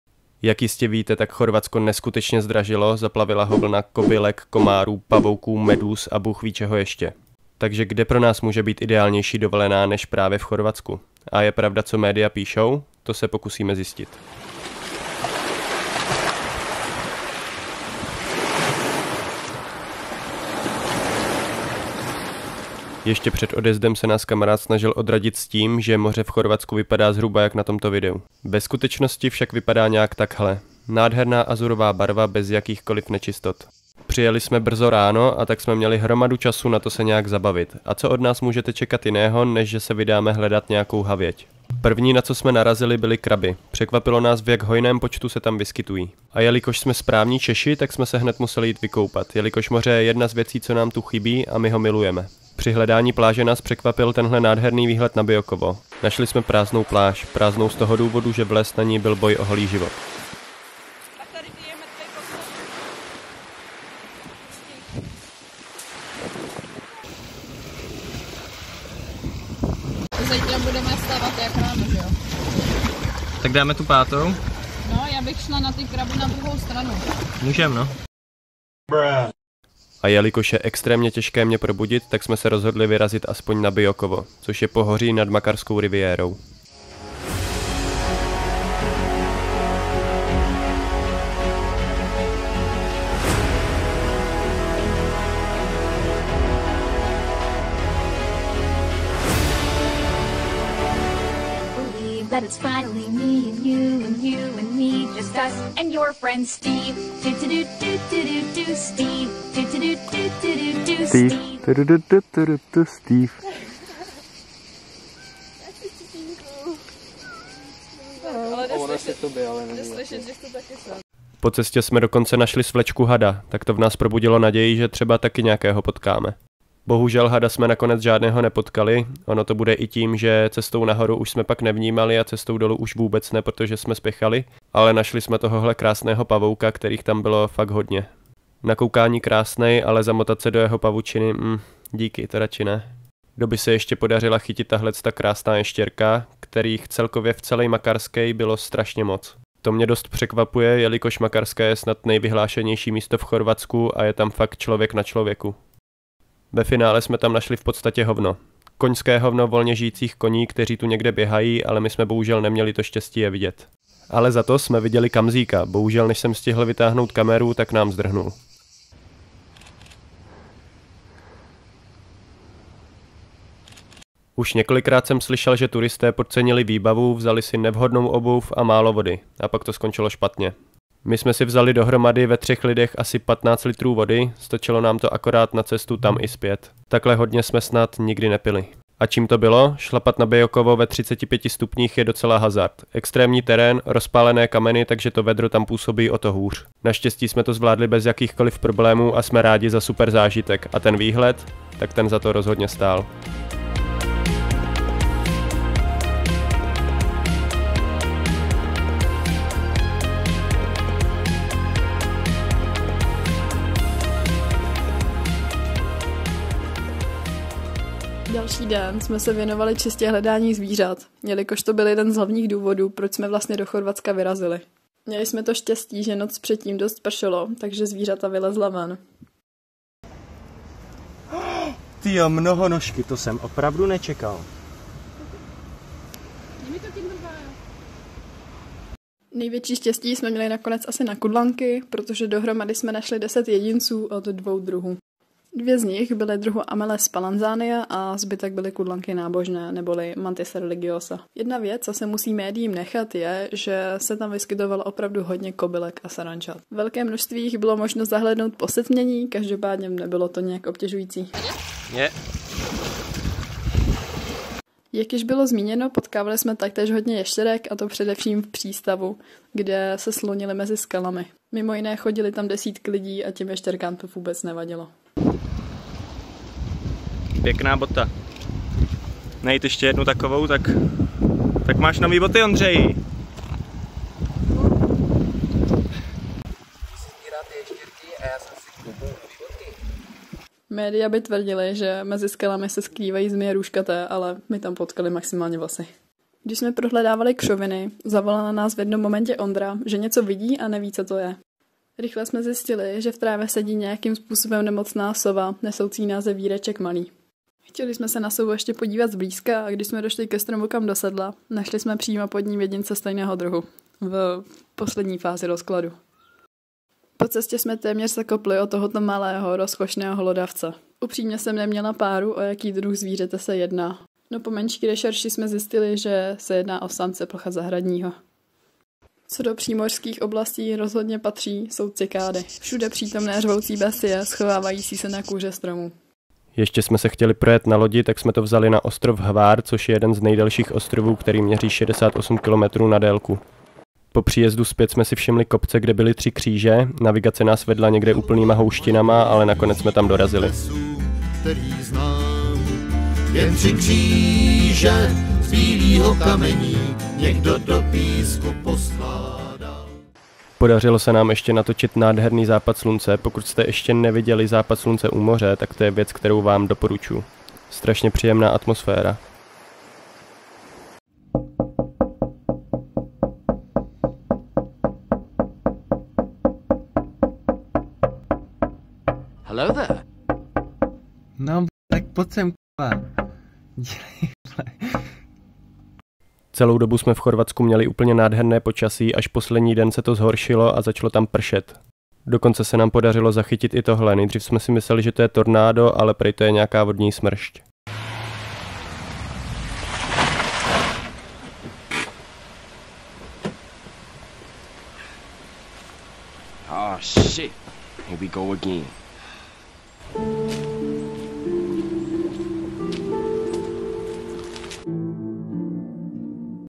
Apokalypsa Kobylek v Chorvatsku?
Je tu dlouho slibované video z Chorvatska, zároveň i takové naše odhalení, společně s Dobi.
Jelo se tam za účelem najít nějaká zajímavá zvířata, to se z části povedlo. Hlavní cíl výpravy bylo najít nějakého hada, to se bohužel nepovedlo. Třeba příště.